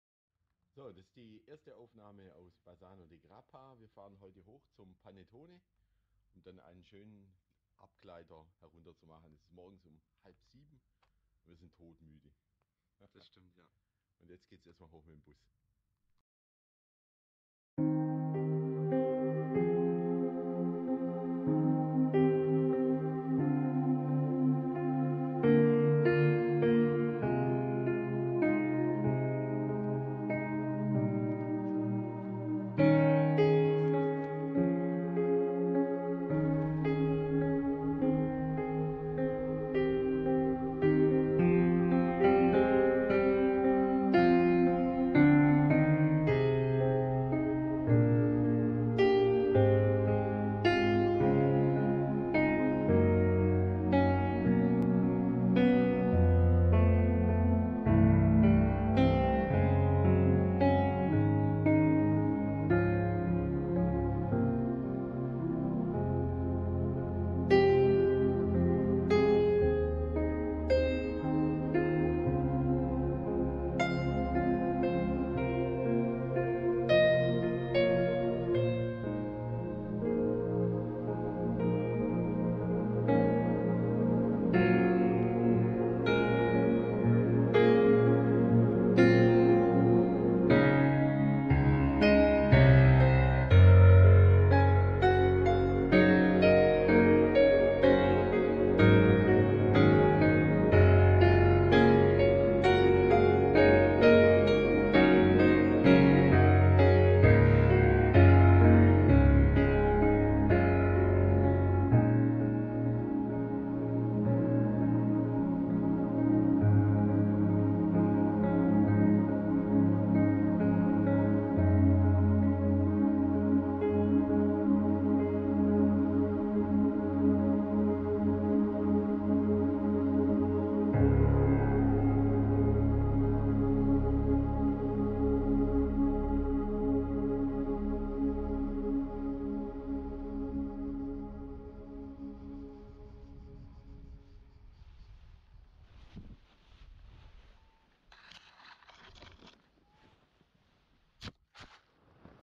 Bassano del Grappa: Paragliding Flug vom Panettone
Ich bin am frühen Morgen mit meinem Gleitschirm vom Panettone nach Semonzo geflogen. Es war ein wunderbarer, ruhiger Flug.